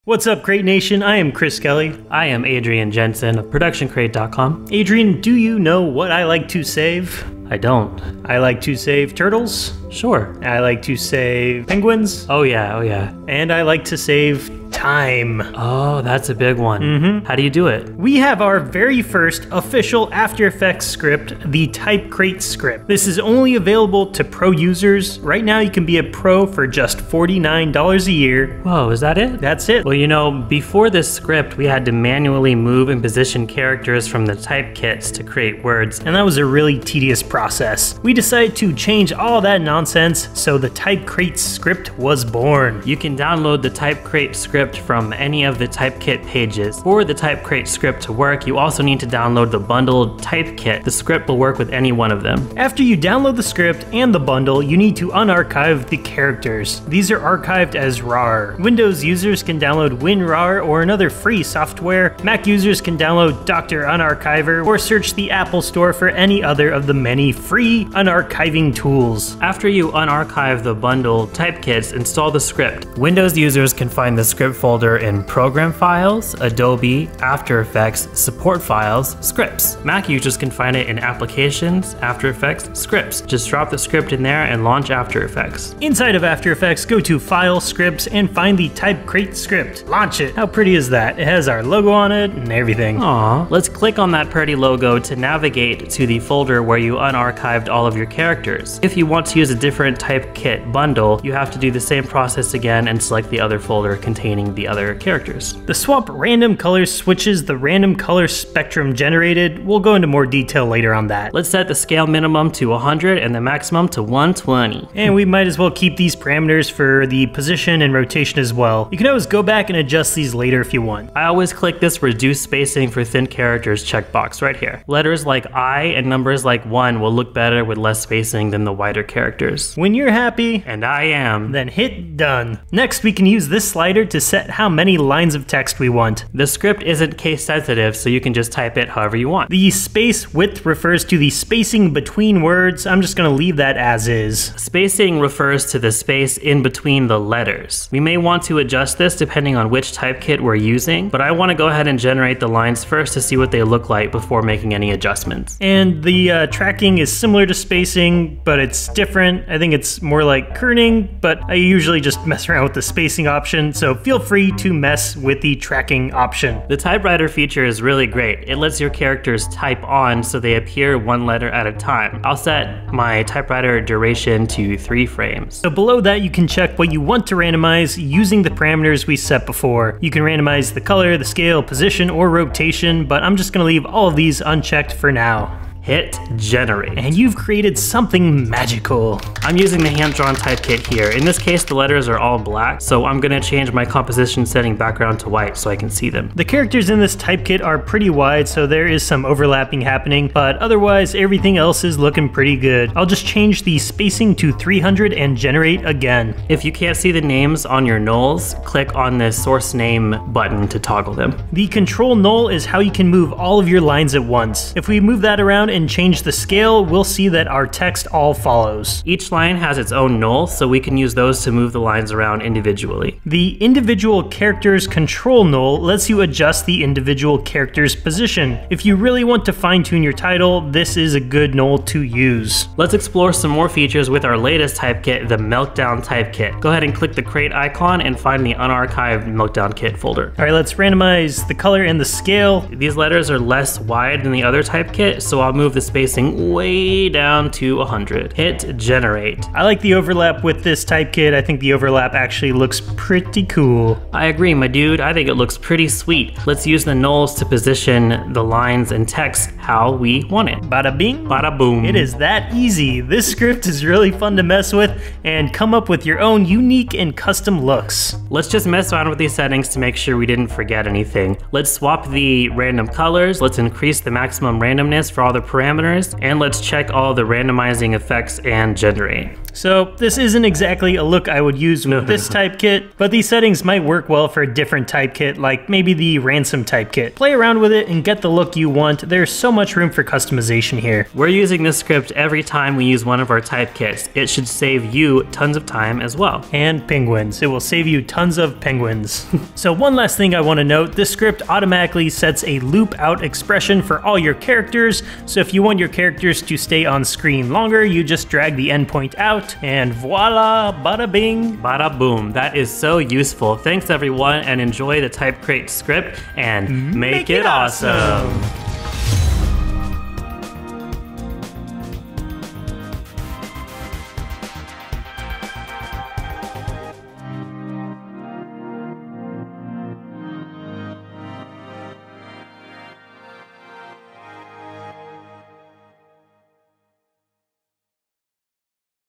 TypeCrate - Animated Type Script for After Effects!
Download the TypeCrate Script from that same page!

This is an After Effects Script available only to Pro Members. This script allows users to type out any words and characters from our existing typekit bundles. We will be releasing more bundles soon!